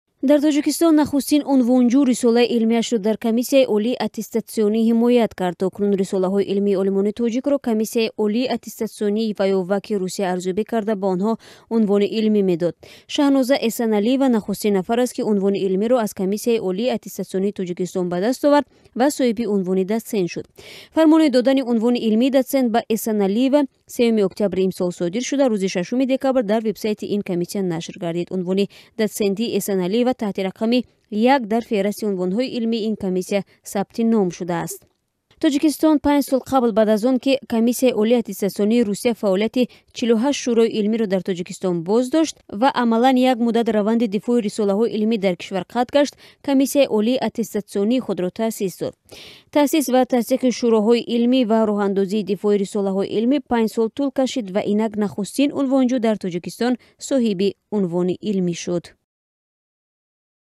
Дифои нахустин рисолаи илмӣ дар ВАК-и Тоҷикистон
Дар Тоҷикистон нахустин унвонҷӯ рисолаи илмиашро дар Комиссияи олии аттестатсионӣ ҳимоят кард. То кунун рисолаҳои илмии олимони тоҷикро Комиссияи олии аттестатсионӣ ваё ВАК-и Русия арзёбӣ карда, ба онҳо унвони илмӣ медод.